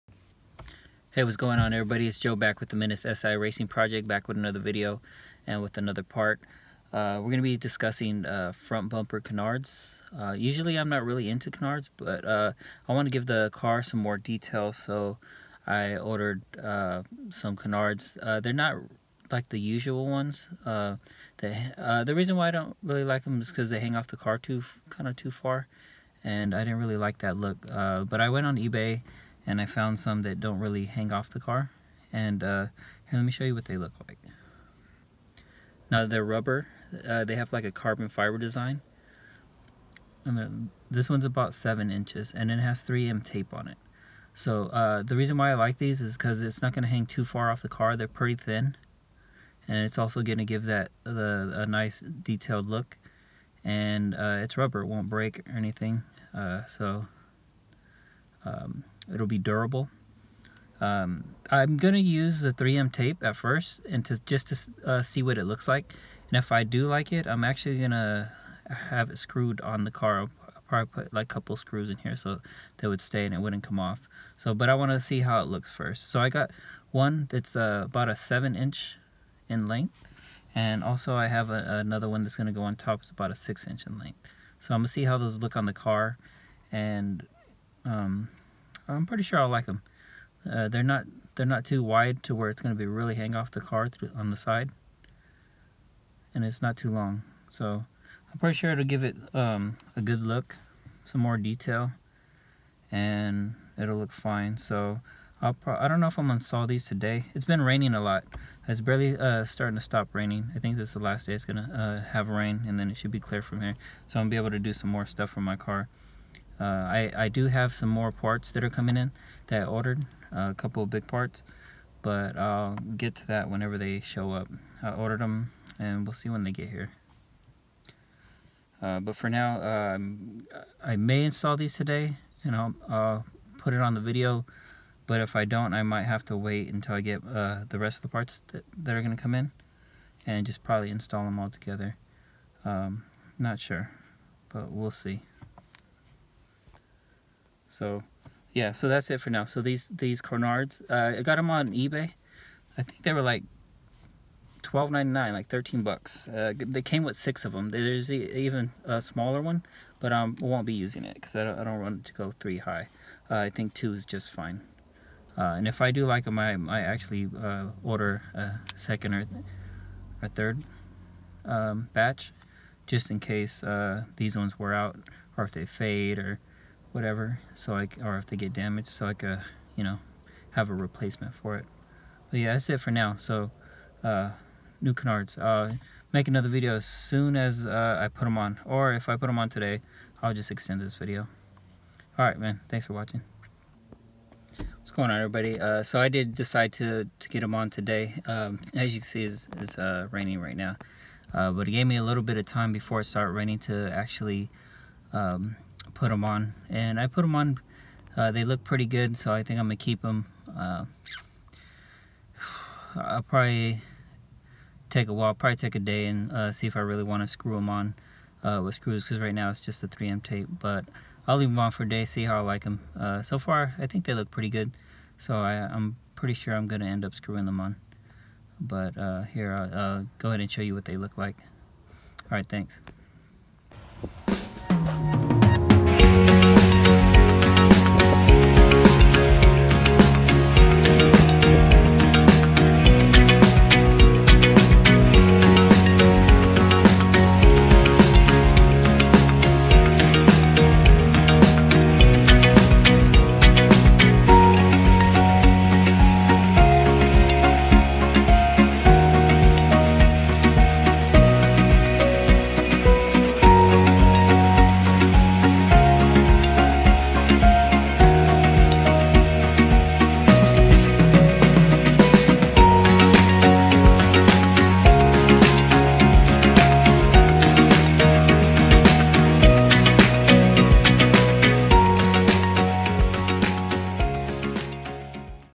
10th Gen Civic Si Coupe canards
Hey what’s going on everybody? In today’s video I will be showing you the new front bumper canards what I installed.